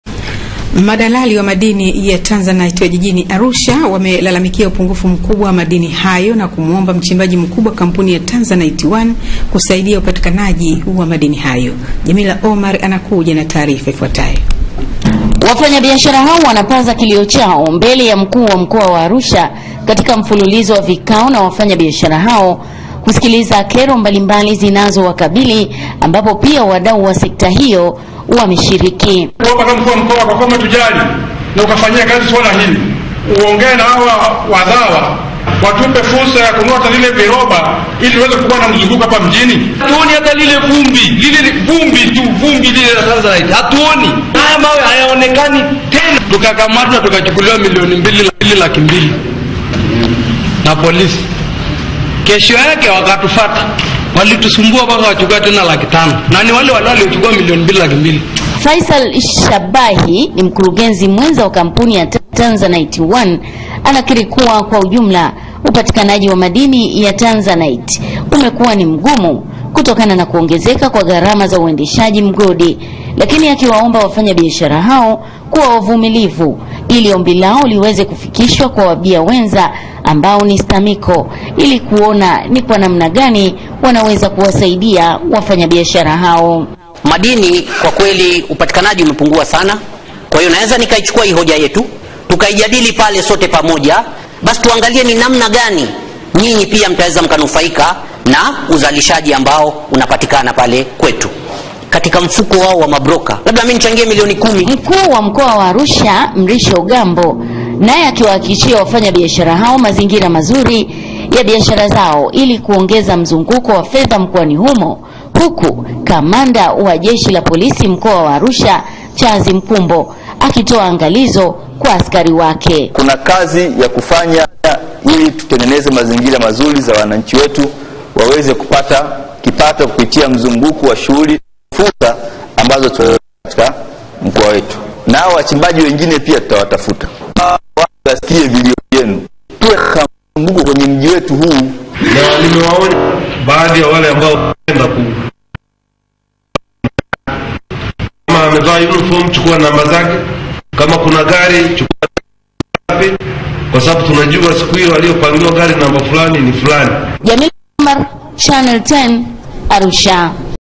Madalali Wa Tanzanite Walia Na Uhaba Wa Madini Hayo
Madalali wa madini ya Tanzanite mkoani Arusha wamelalamikia upungufu mkubwa wa madini hayo na kuomba kampuni ya uchimbaji wa madini kusaidia upatikanaji wa madini hayo.